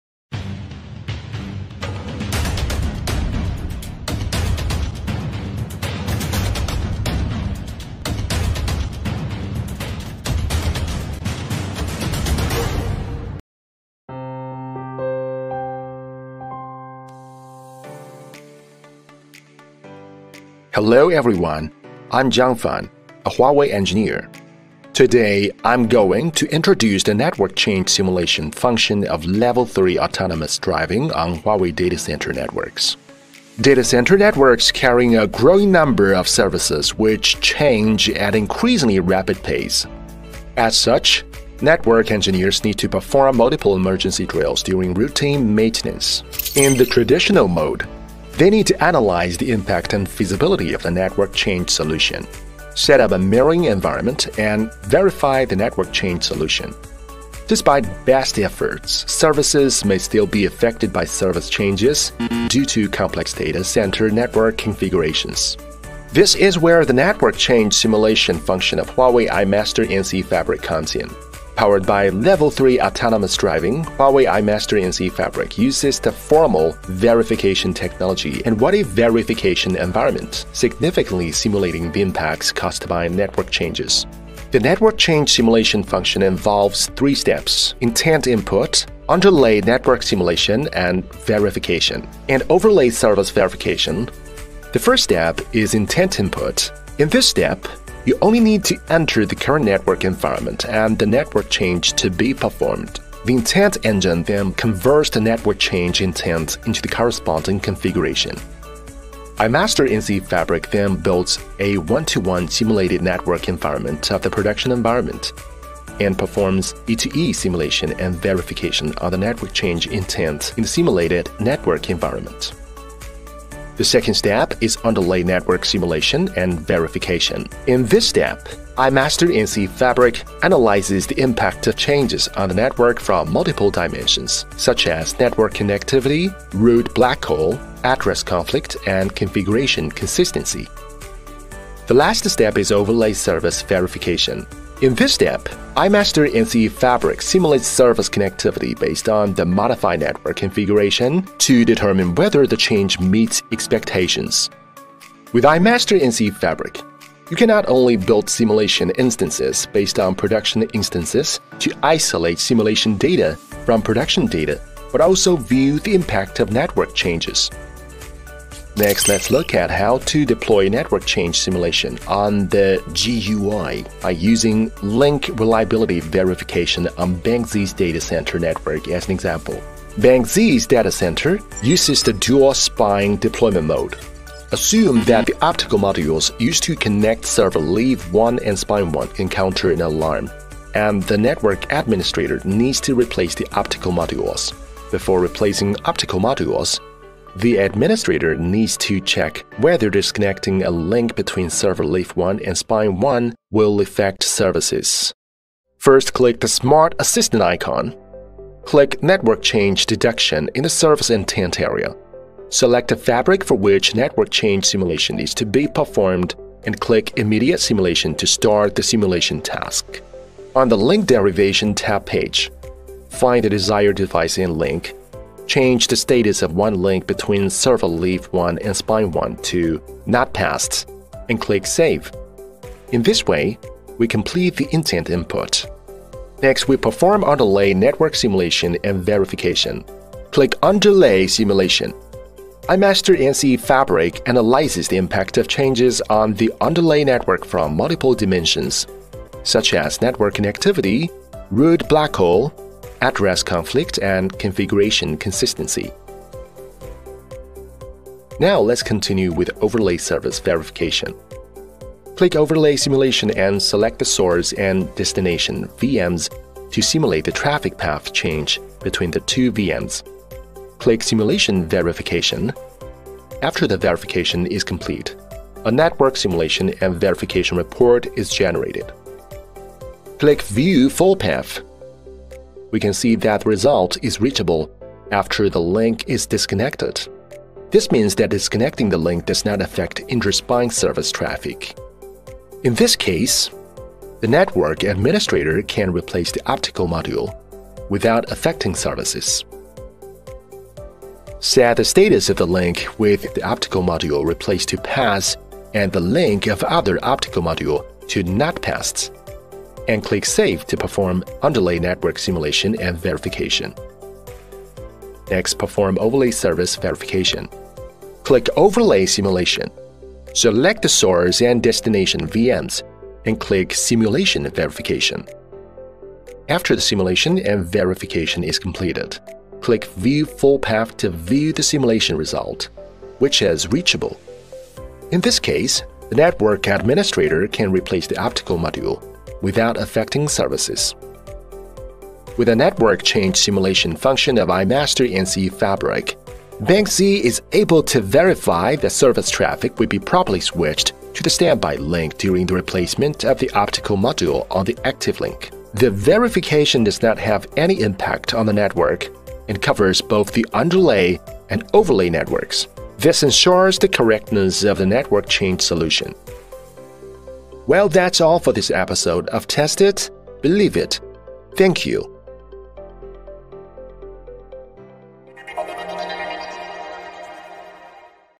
Data Center Networks ― Autonomous Driving Network: Network Change Simulation
This video introduces the network change simulation function of Huawei's iMaster NCE-Fabric. With this function, user is able to verify that service traffic will be properly switched to the standby link during replacement of the optical module on the active link. The verification does not have any impact on the network and covers both the underlay and overlay networks. This ensures the correctness of the network change solution.

For details about the principles and examples of the network change simulation function provided by Huawei iMaster NCE-Fabric, see: